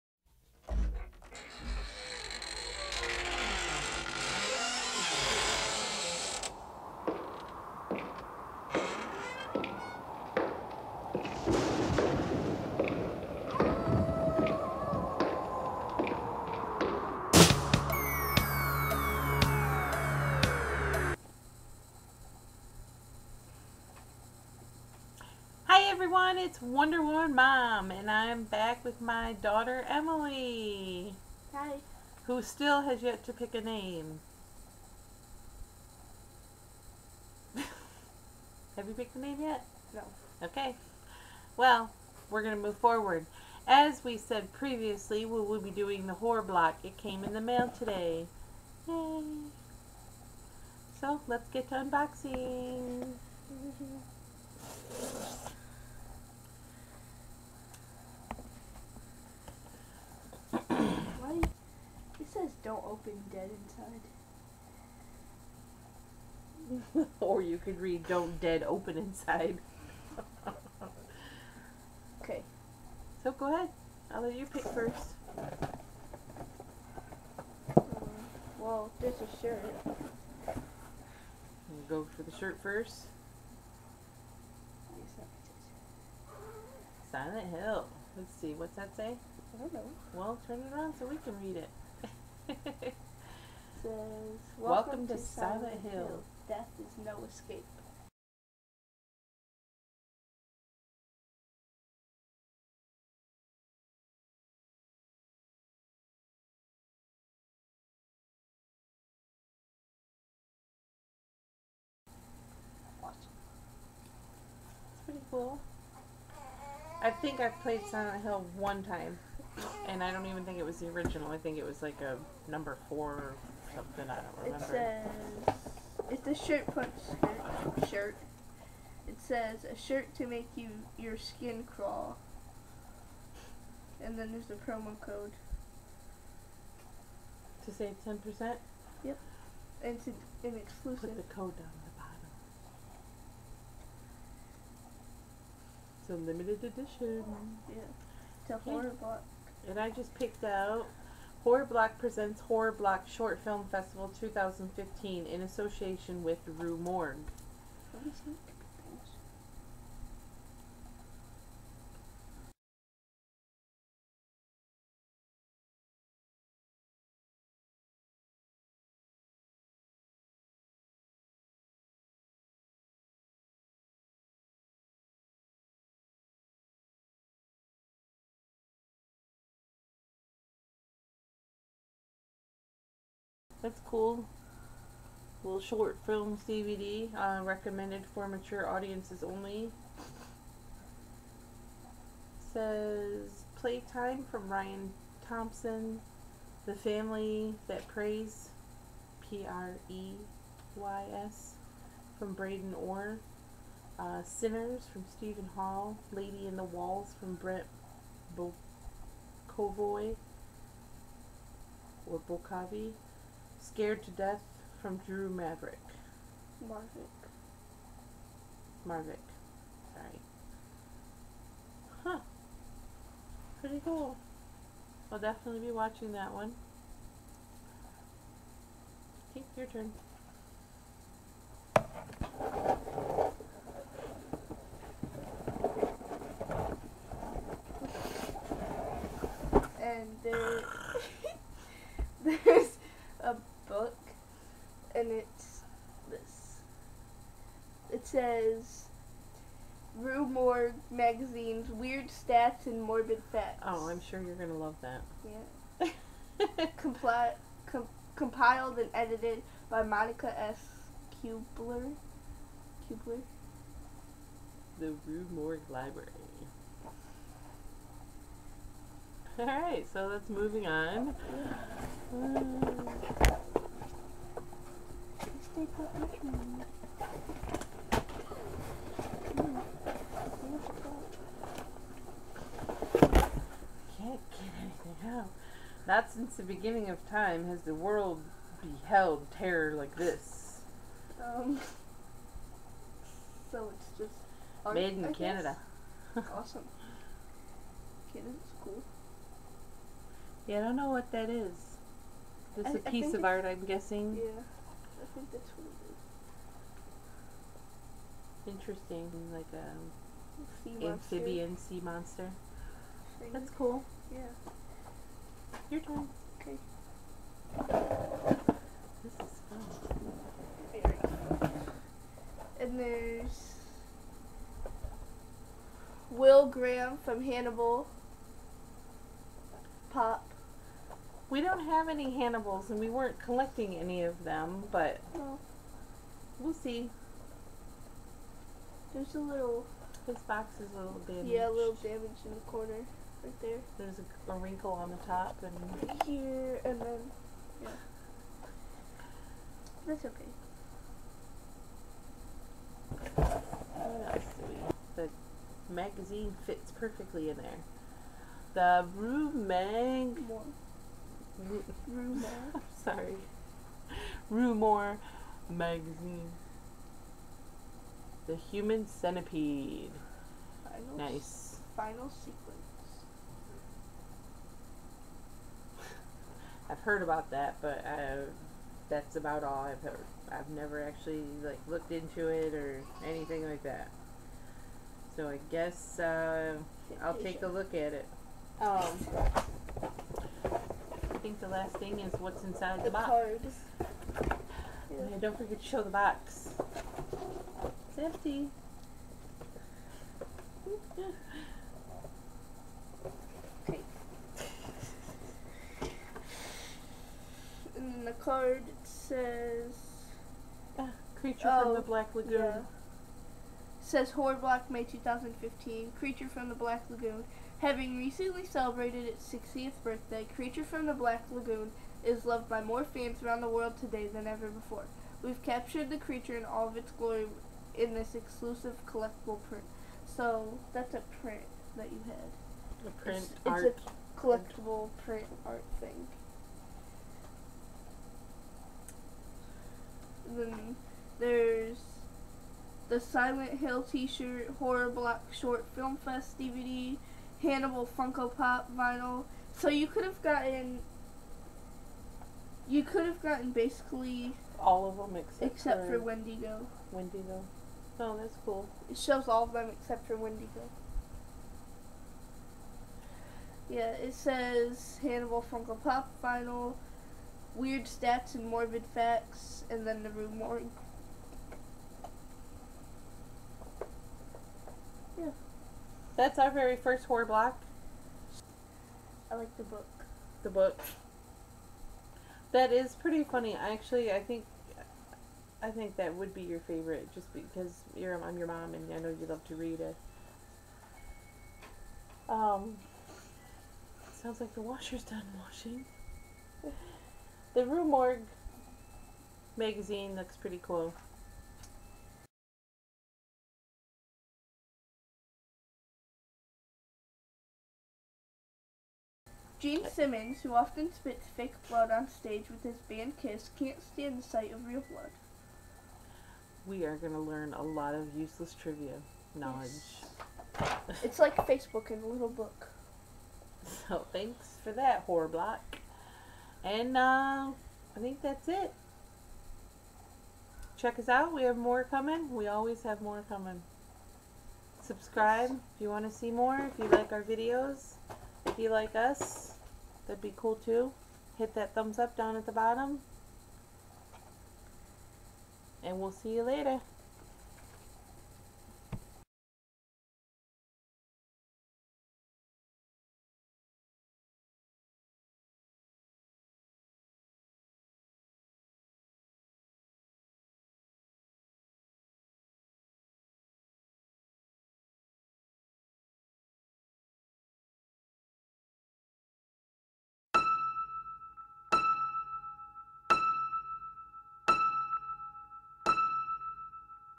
Horror Block May 2015
Welcome to my unboxing of the May 2015 Horror Block!
Fast-Forwarding is no escape!
(play on Silent Hill. )
With special guest Geeky Horror Girl!!

We are extremely sad to tell you that the Shirt Punch Code was misplaced before we uploaded the video. We are very sorry!